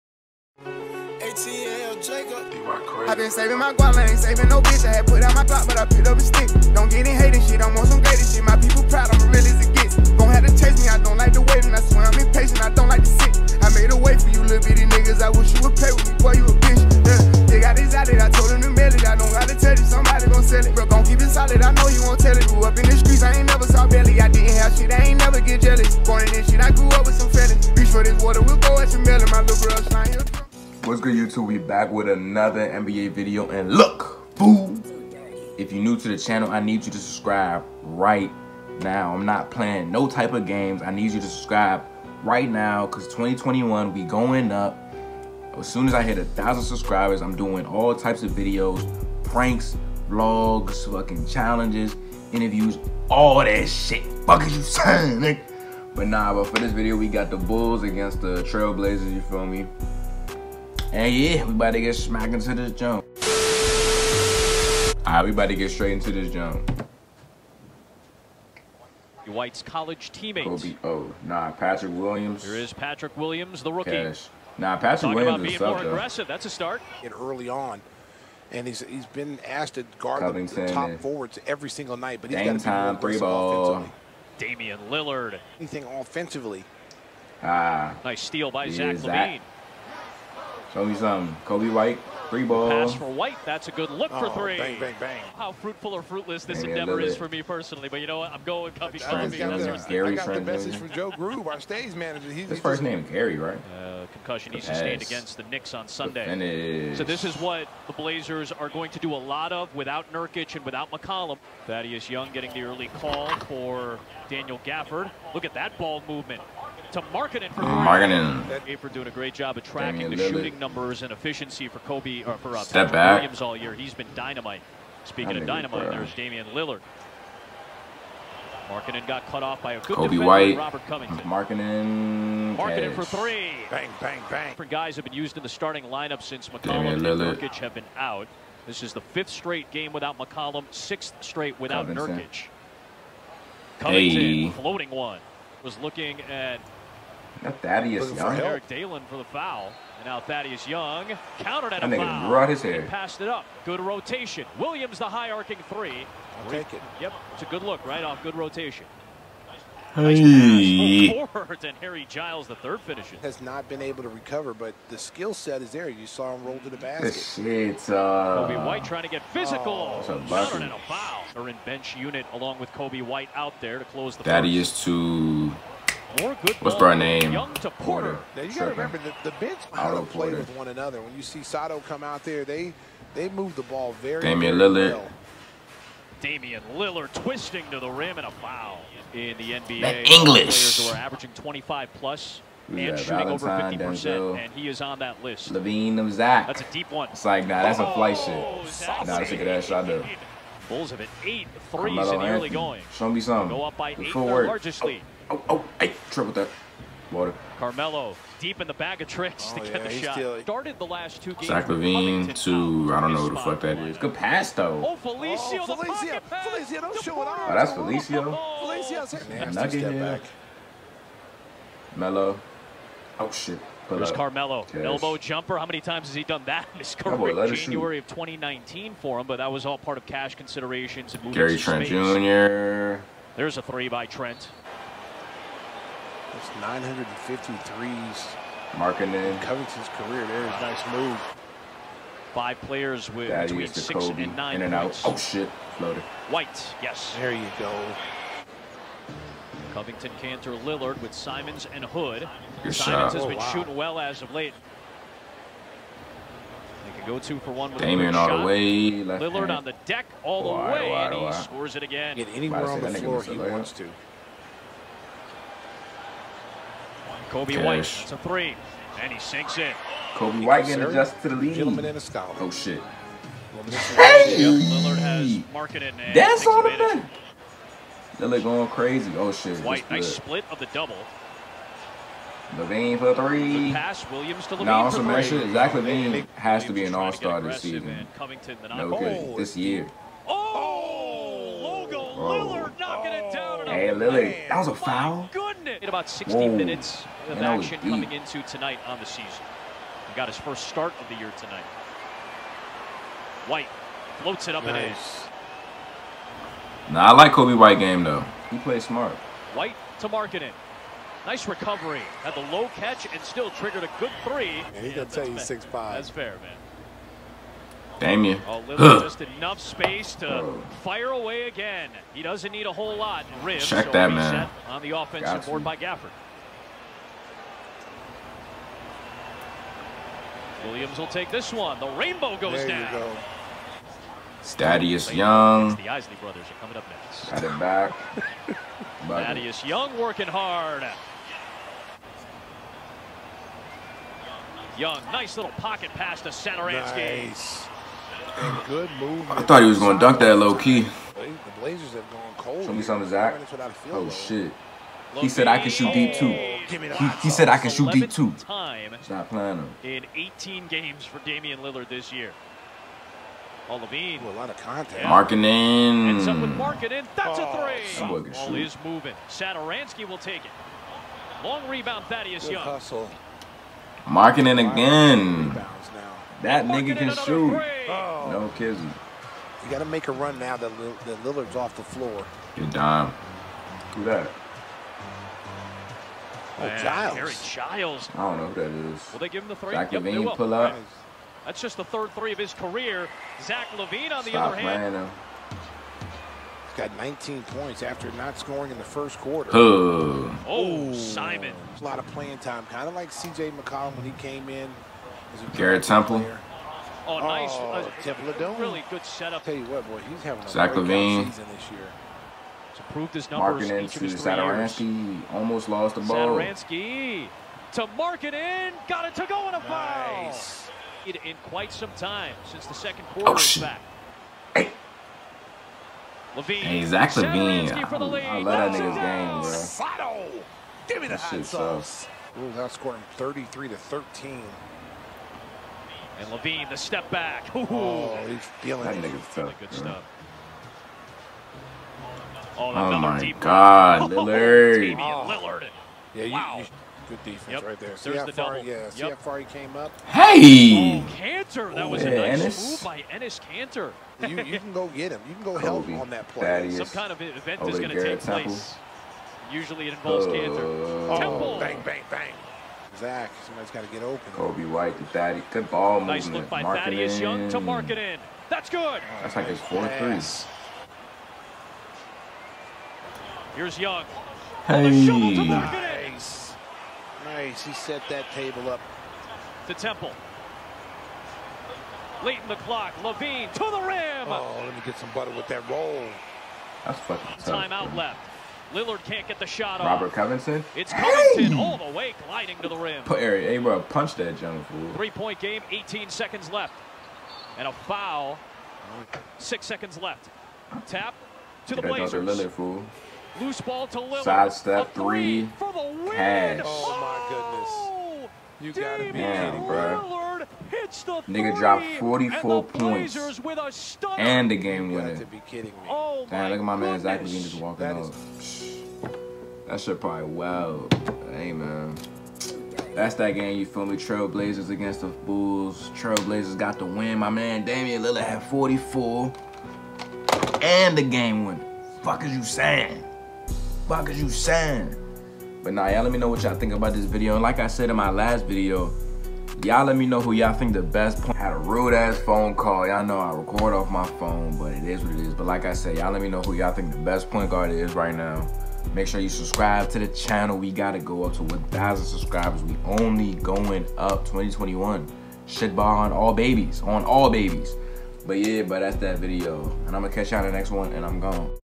TRAIL BLAZERS VS BULLS FULL GAME HIGHLIGHTS🔥| DAMIEN LILLARD DROPS 44PTS + GAME WINNING SHOT😱
COMMENT ANYTHING U WOULD LIKE ME TO WATCH‼️

@_djwain_      - INSTAGRAM
@_DJ133_      - TWITTER
@Tkilla1335   - SNAPCHAT
@dj_wain_      - TWITCH